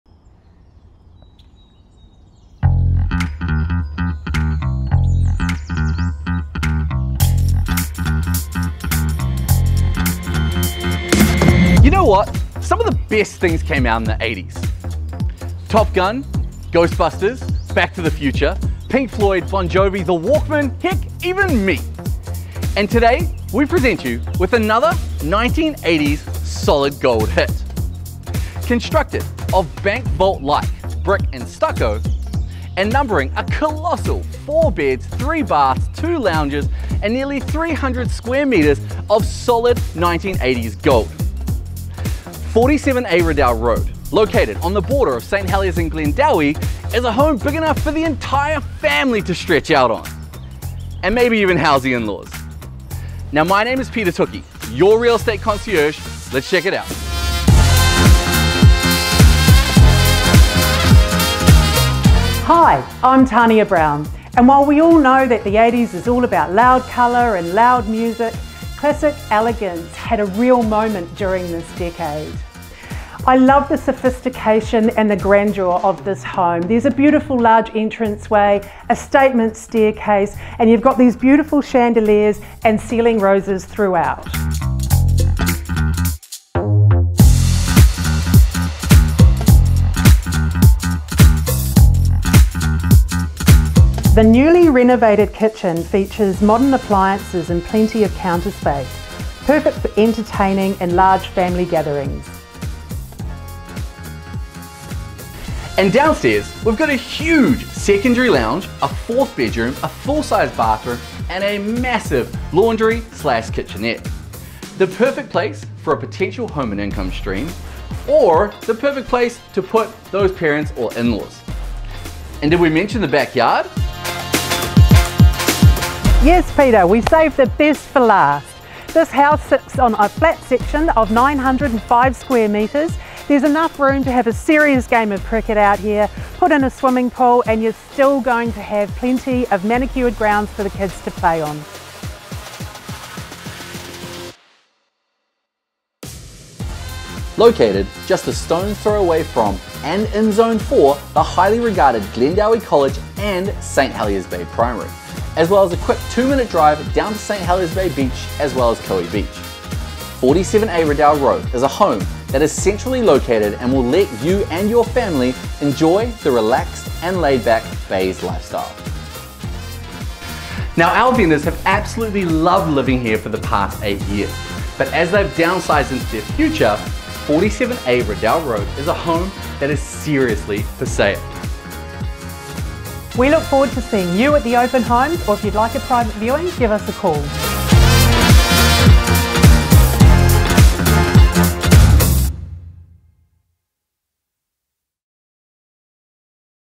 SOLD - 47A Riddell Road, Glendowie - Tania Brown and Peter Tuki
Priced to Sell!  Almost $1 Million UNDER 2021 CV!

This has to be one of (if not) the best buys in the Bays!
Our vendor has bought and has instructed to present all offers.

This will not last.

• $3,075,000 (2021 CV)
• Freehold 905m2 ...

Property Marketed by: Tania Brown, Mission Bay and Peter Tuki, St Heliers